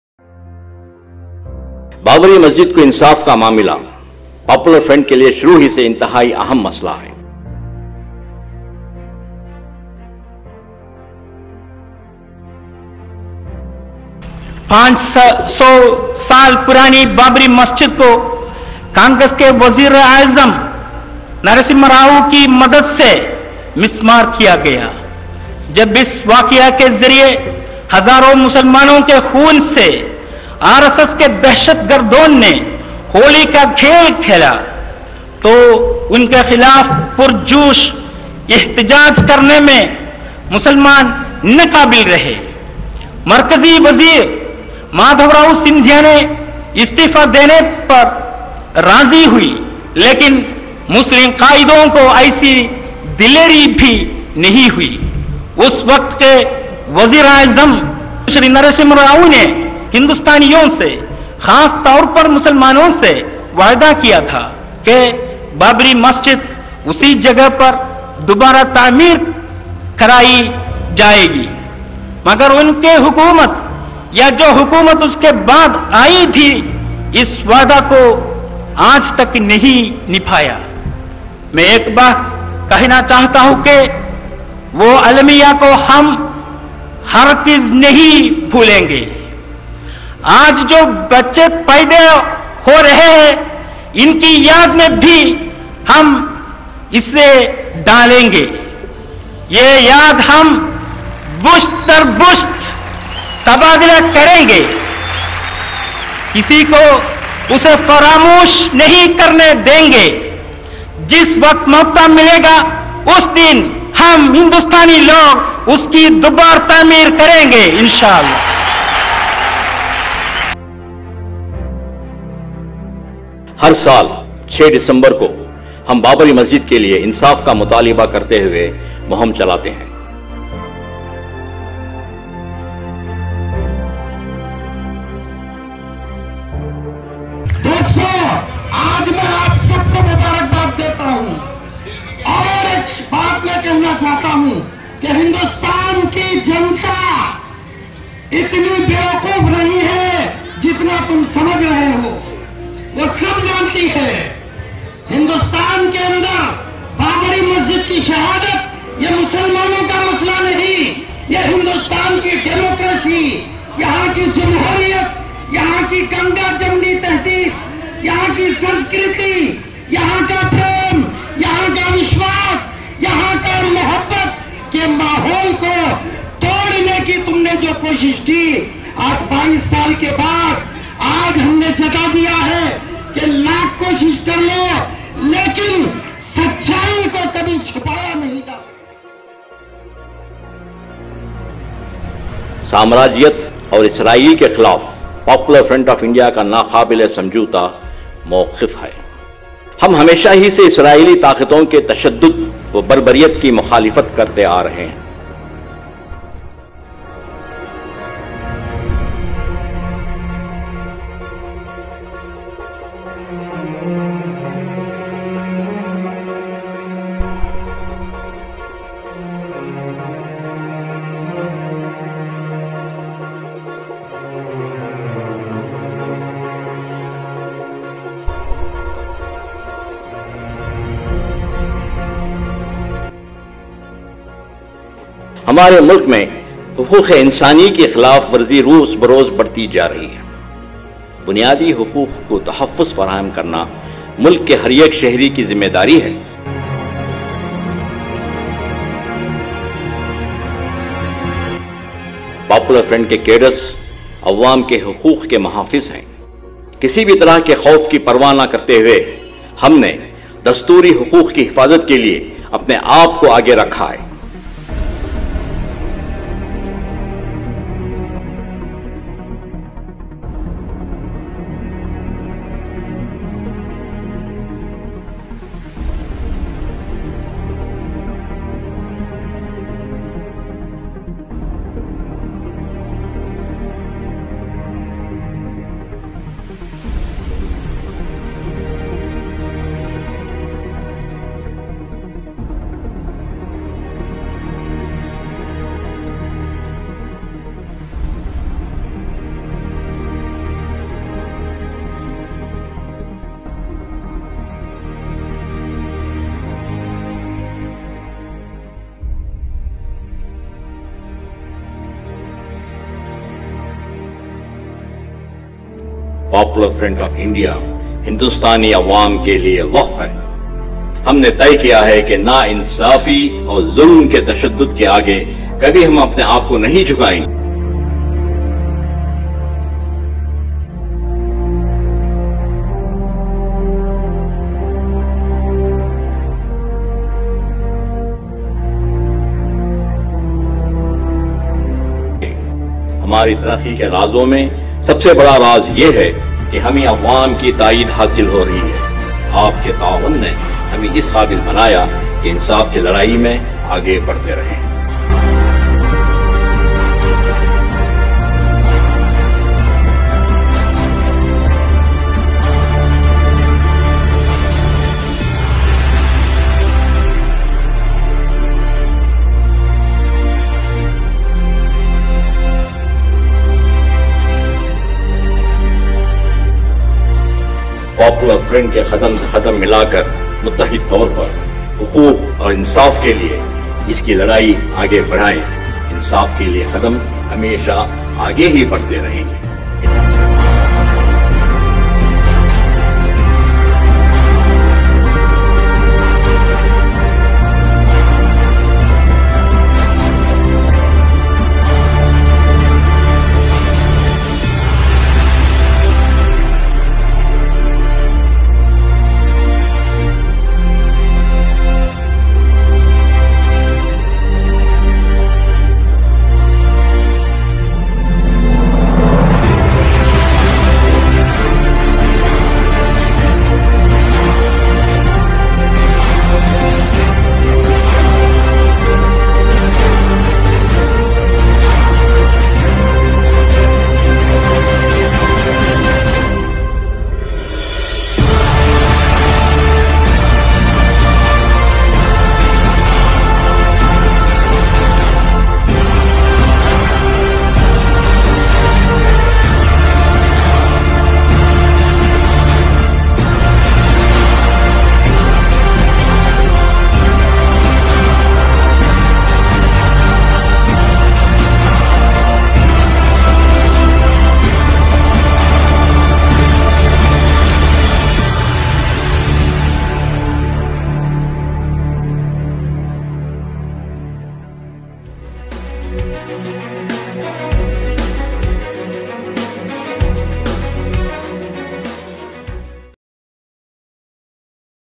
Popular Front Of India Documentary Film
Popular Front of India it is Movement. struggling for Minorities, Daliths, & Aadiwasis  Empowerment in India.